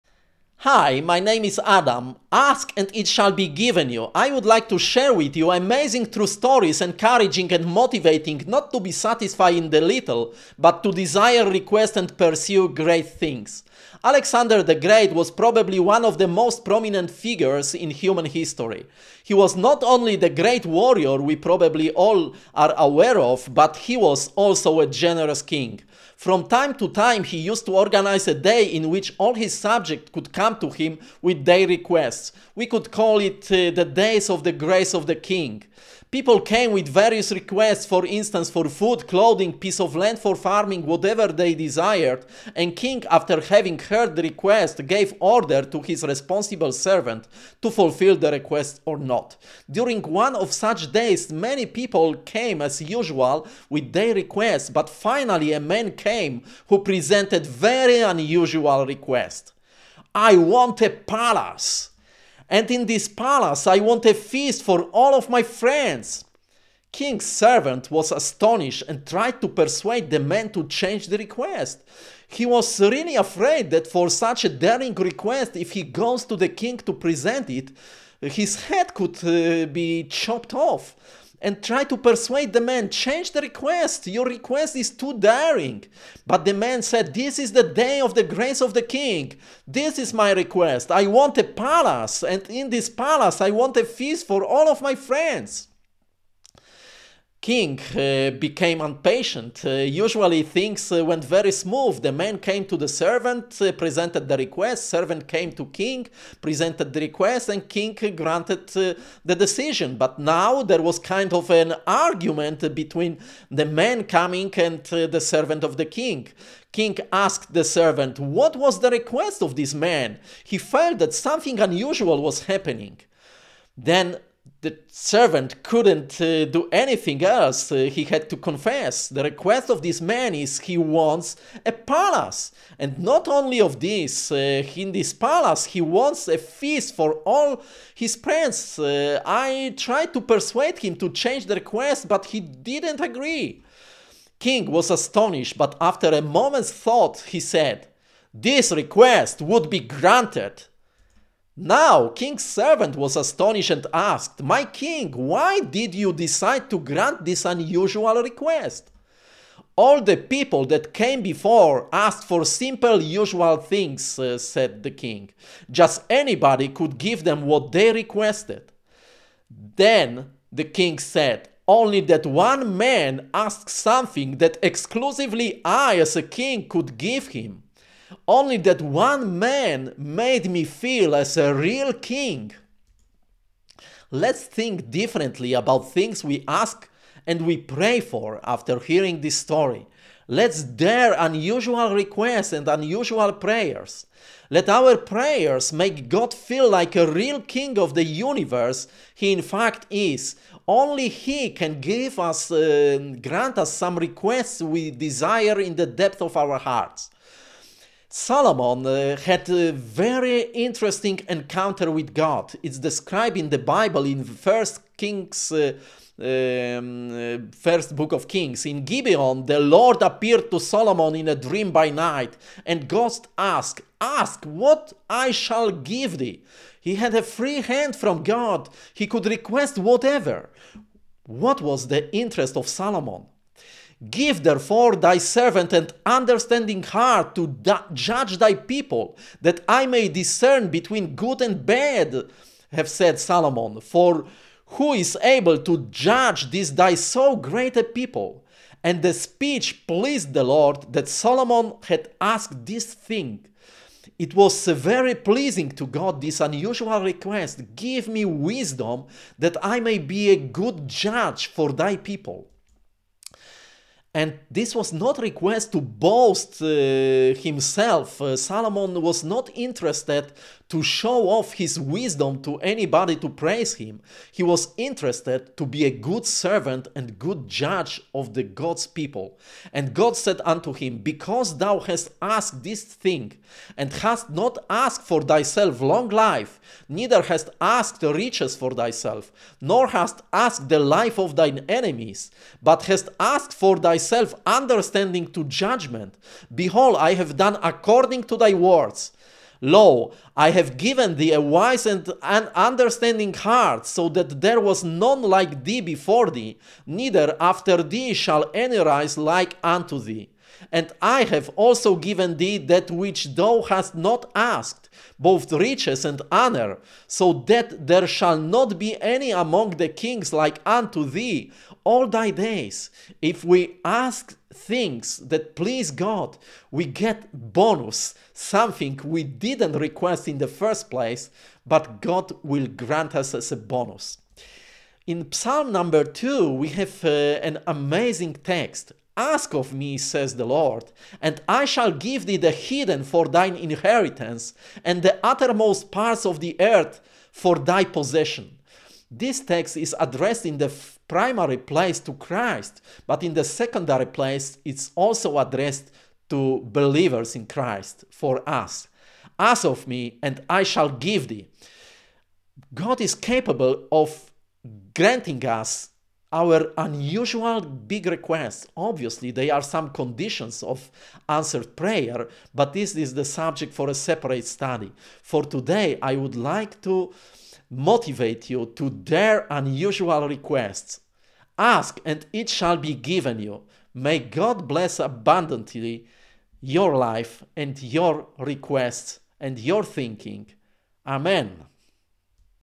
Ask, and it shall be given you
Two amazing and motivating true stories, as illustration that it is worth to ask and pray -  especially for great things, which are important to us, even if they seem impossible to be obtained. Illustration of power of prayer. One story is from life of Alexander the Great and a person who presented to him very daring request, and the second one from one of the most prominent biblical figures and his encounter with God.